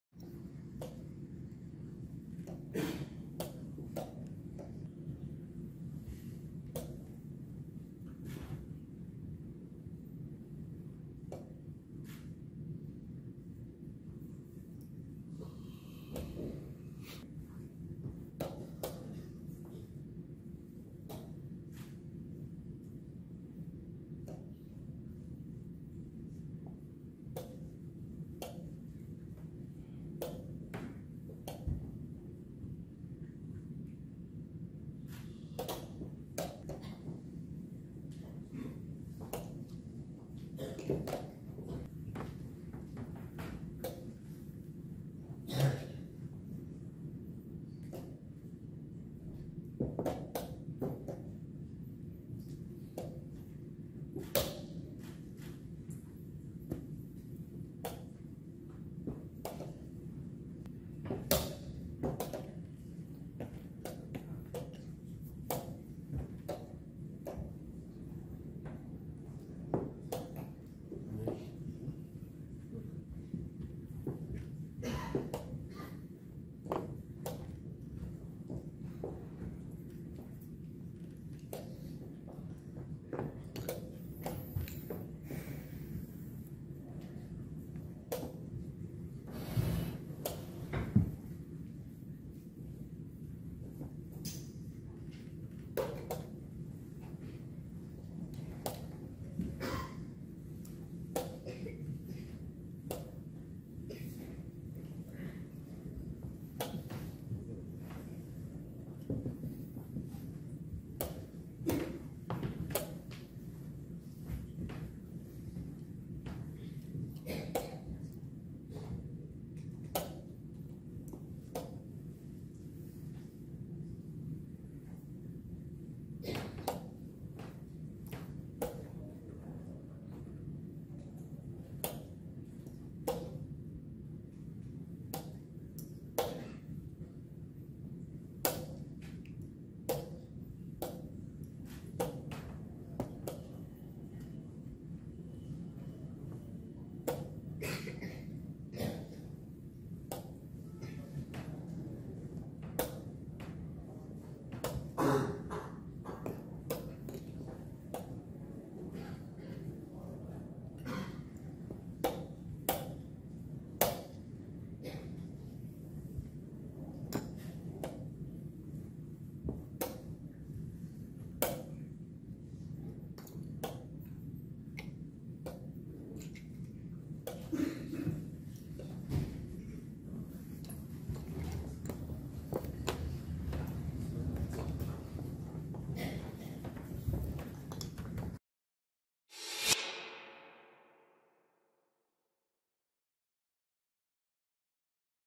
Passive Play leads to one-sided Domination - IM Lisandra v WIM Nimmy
WIM Nimmy A G (2057) defeated IM Lisandra Teresa Ordaz Valdes (CUB, 2295) in their first Blitz game at Che International Chess Festival 2023. Can Nimmy repeat the same result?